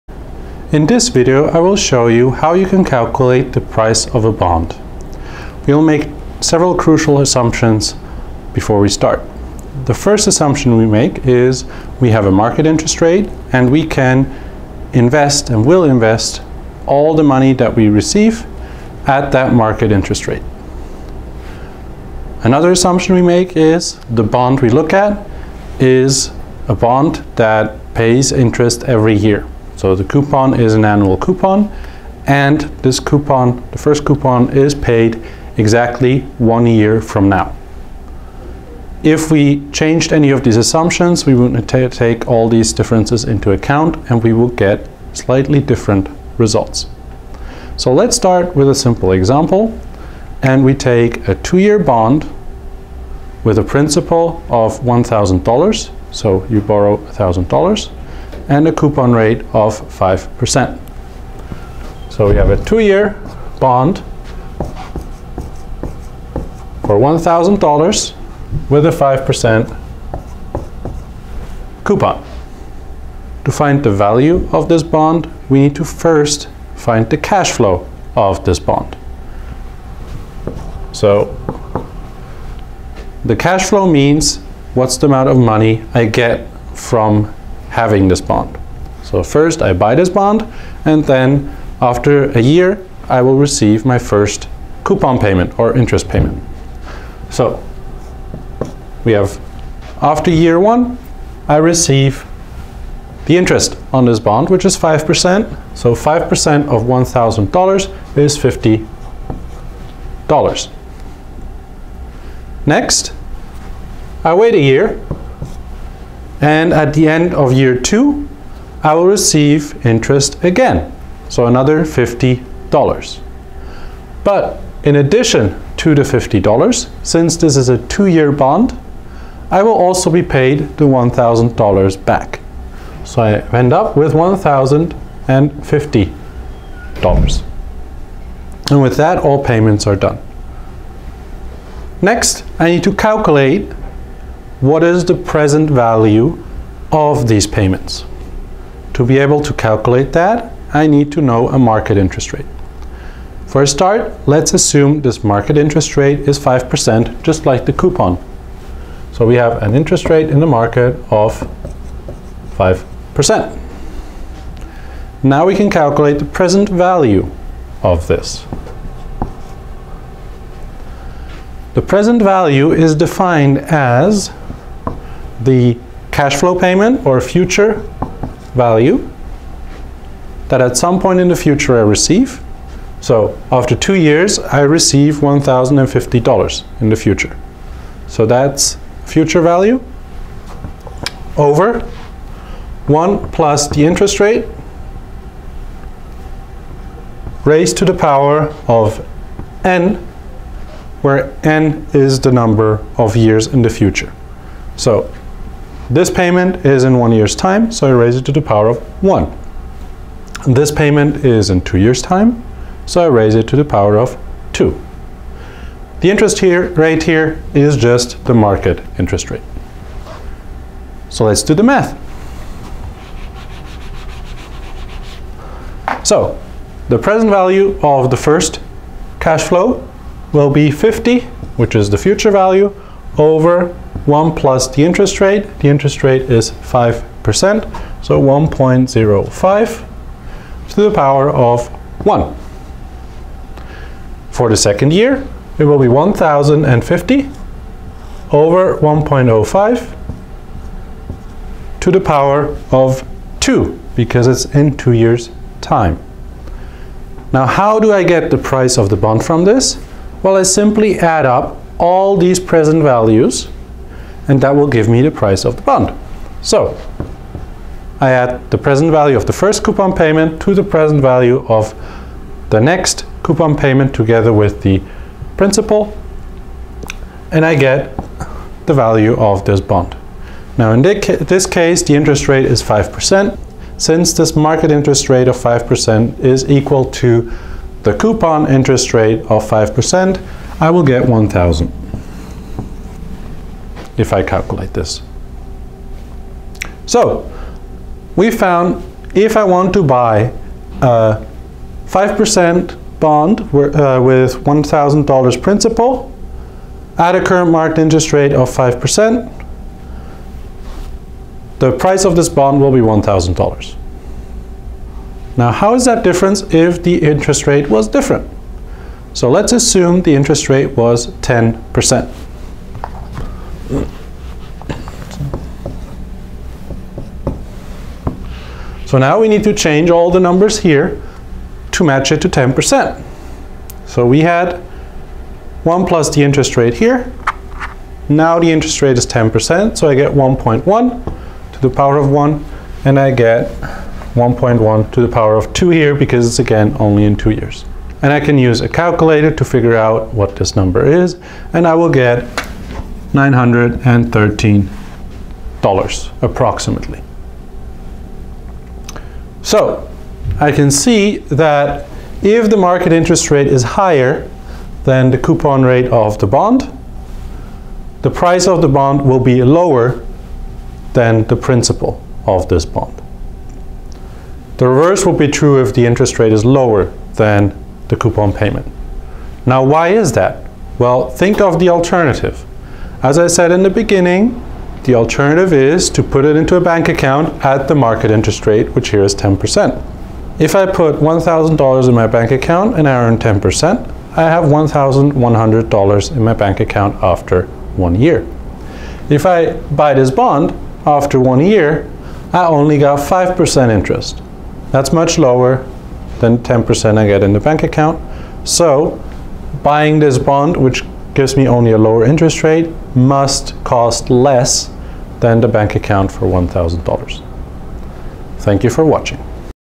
Calculating the price of a bond
This video shows how the price of a bond can be calculated using the present value formula.